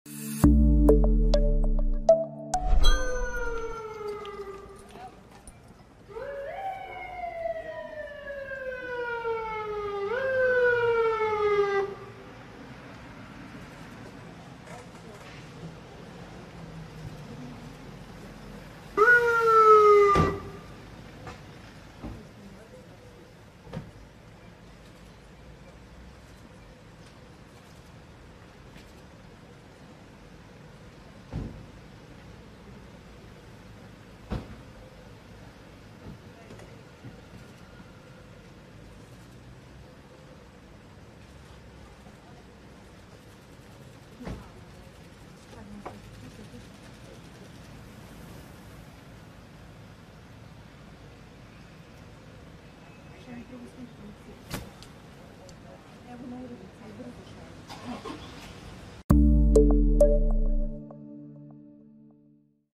BELI, ZAŠTITNI KOMBINEZON, NA NOGAMA SAMO ČARAPE Uroš Blažić doveden u policijsku stanicu
Uroš Blažić, osumnjičeni za pucnjavu u okolini Mladenovca u kojoj je ubijeno 8 osoba, a povređeno 14 osoba, doveden je u PU Smederevo.